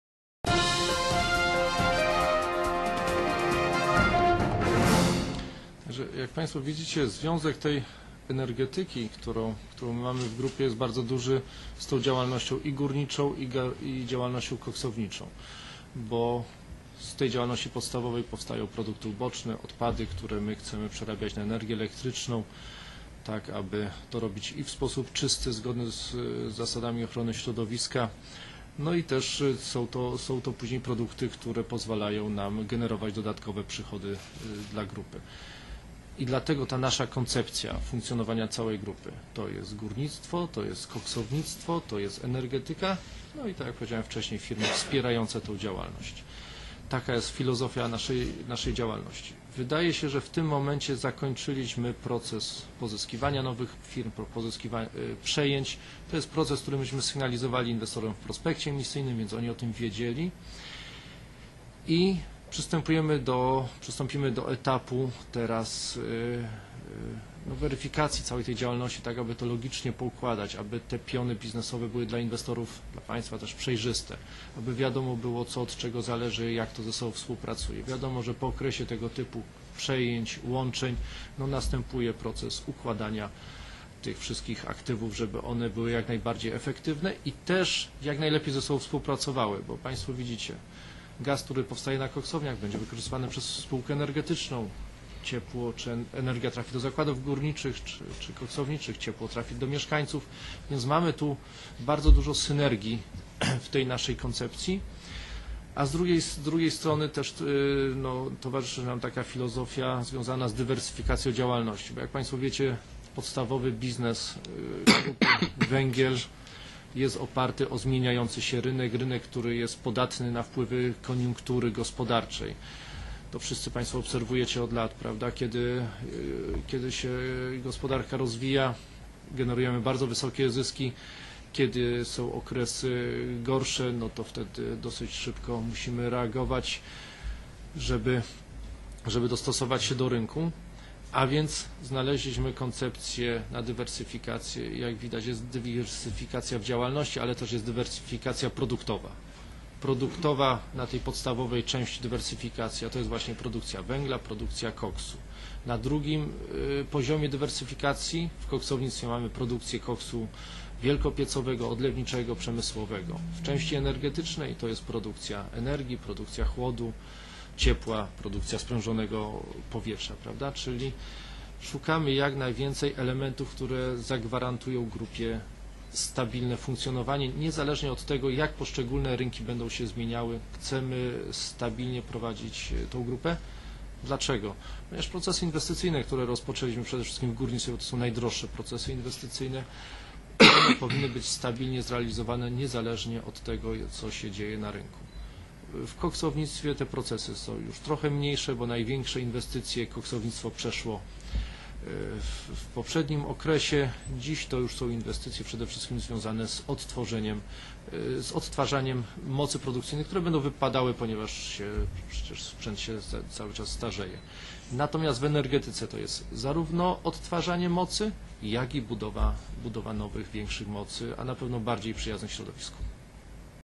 Jarosław Zagórowski o grupie kapitałowej JSW
Jastrzębie, 6 października 2011 roku. Konferencja prasowa Jastrzębskiej Spółki Węglowej.
Realizacja Jarosław Galusek - nettg.pl